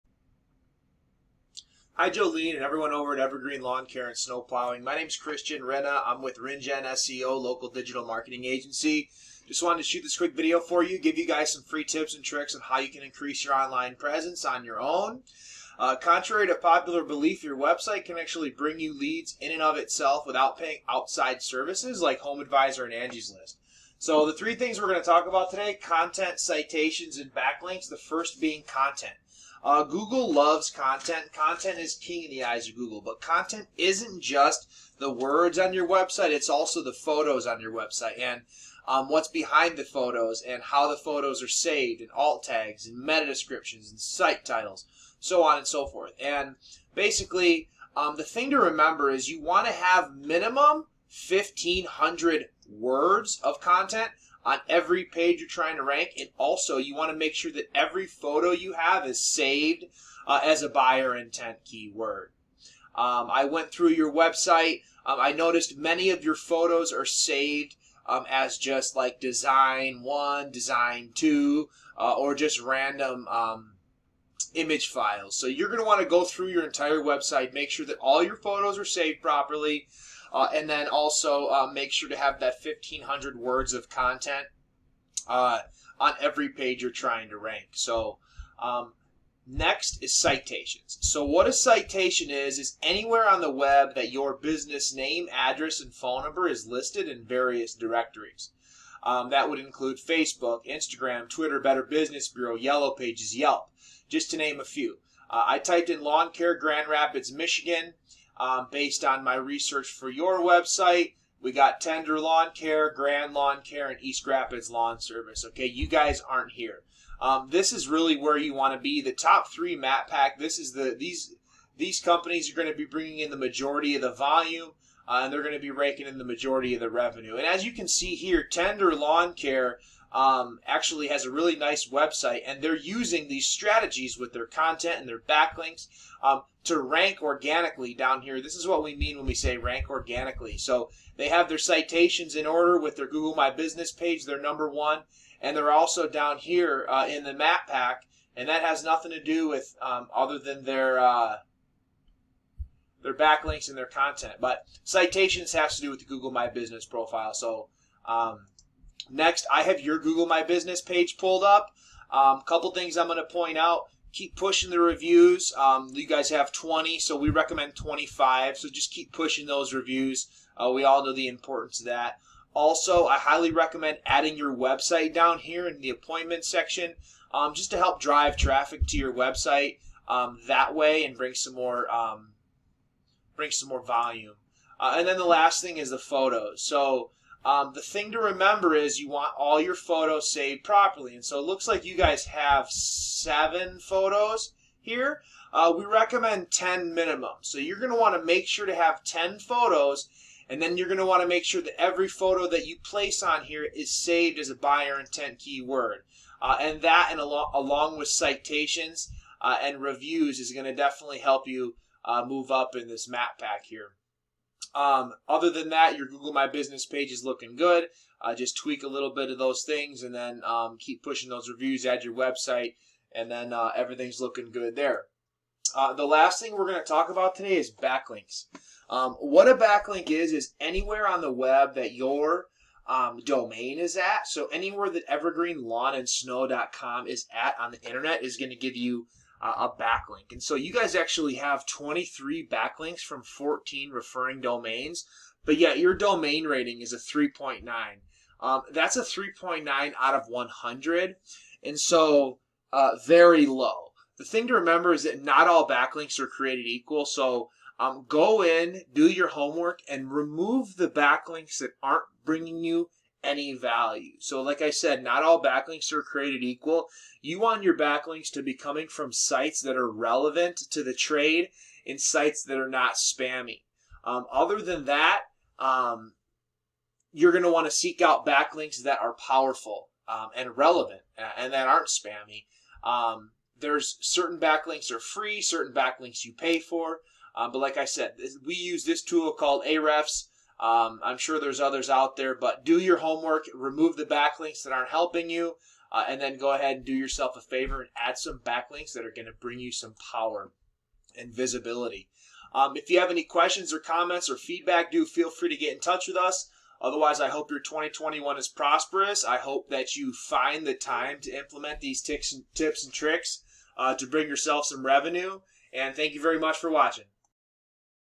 Evergreen Lawn Care & Snowplowing - Website Review
How to increase your online presence with these few tips and tricks!